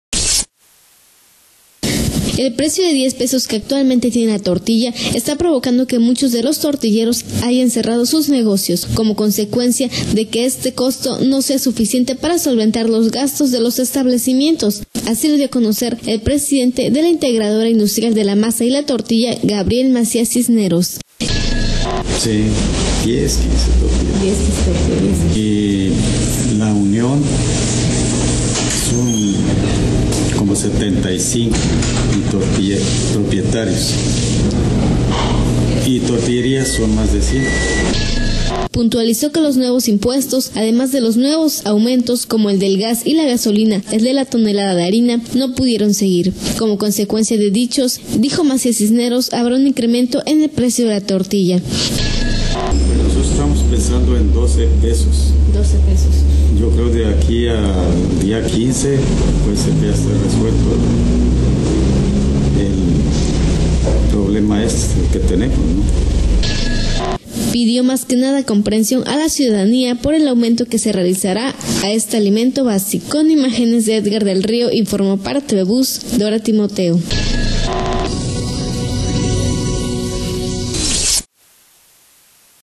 Cierran entre 10 y 15 tortillerías, por bajo costo de tortilla
El precio de 10 pesos que actualmente tiene la tortilla, está provocando que muchos de los tortilleros hayan cerrado sus negocios, como consecuencia de que este costo no sea suficiente para solventar los gastos de los establecimientos, así lo dio a conocer el presidente de la integradora industrial de la masa y la tortilla Gabriel Macías Cisneros